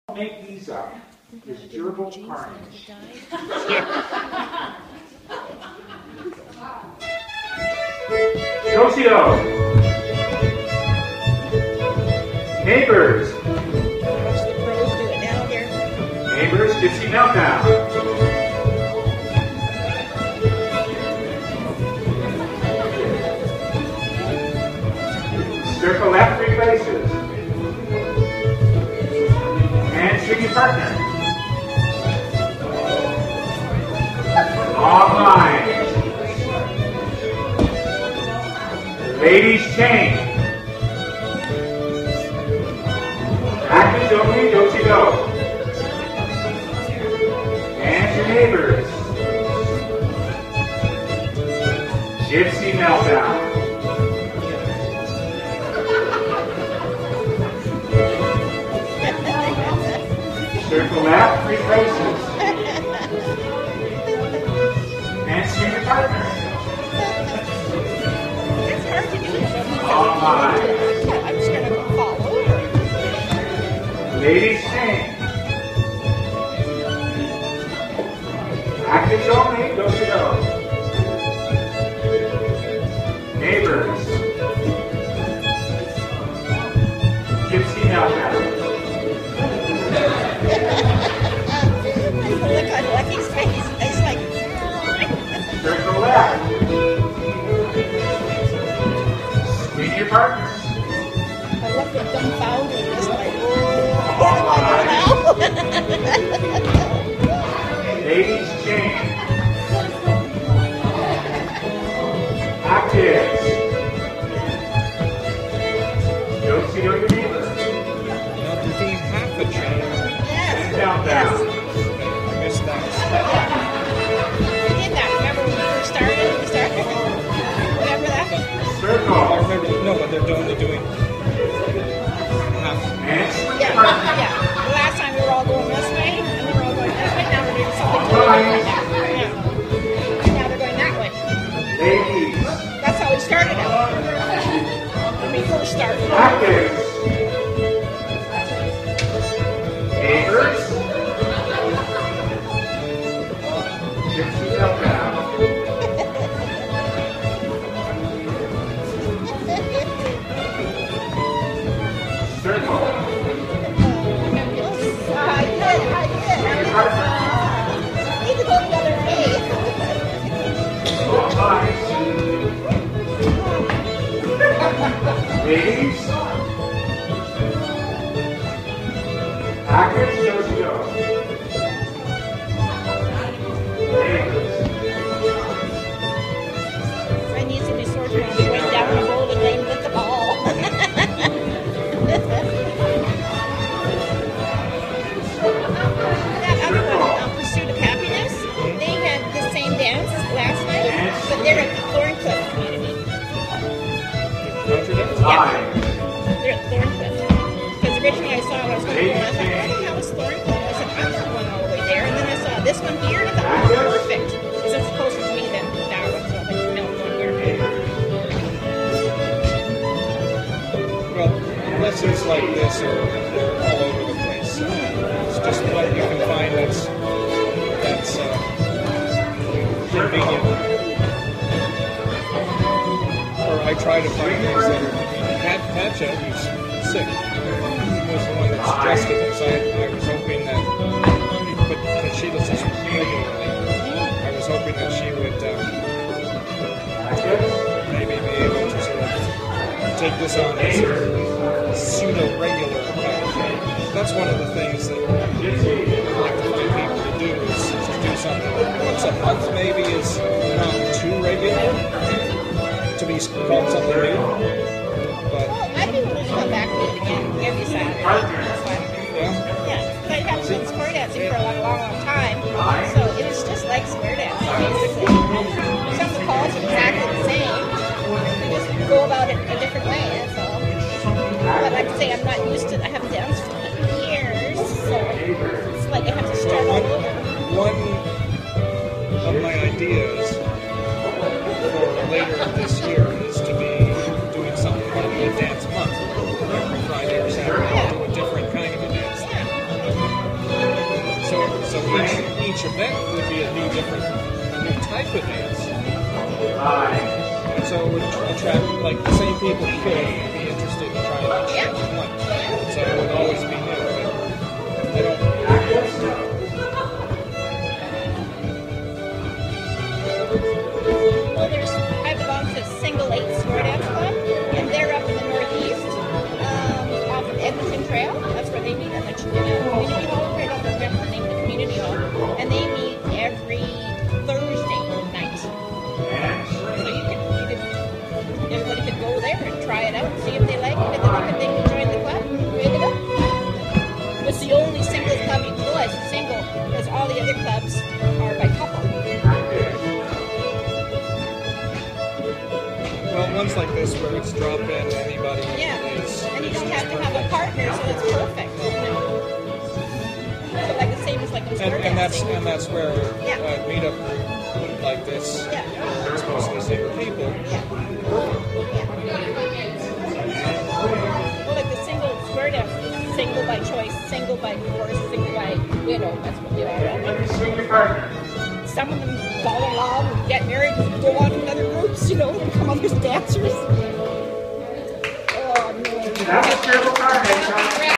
2016 02 06 Calgary Contra Dance
DSN went to the Calgary Contra Dance group's February event, learned a few steps & had a few laughs!